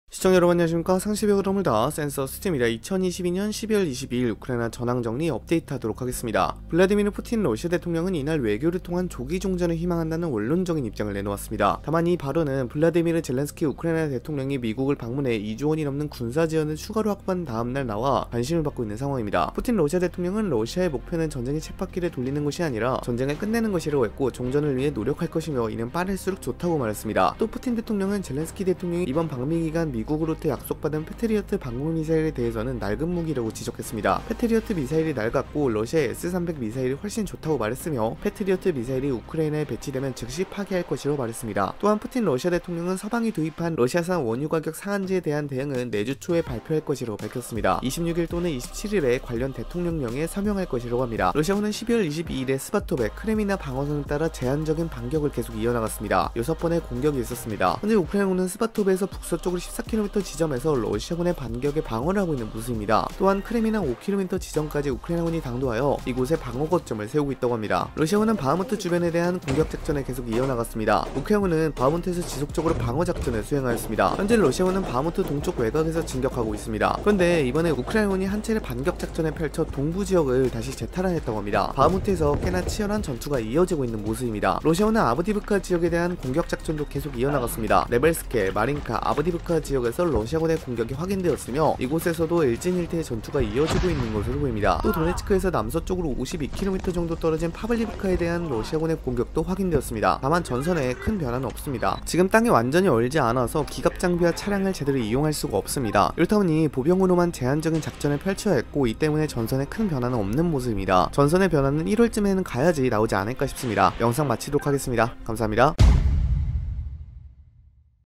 우크라이나 전쟁 전황 정리 12월 22일 - 푸틴 "조기 종전 희망"
우크라이나 전쟁의 12월 22일자 전황 정리 영상입니다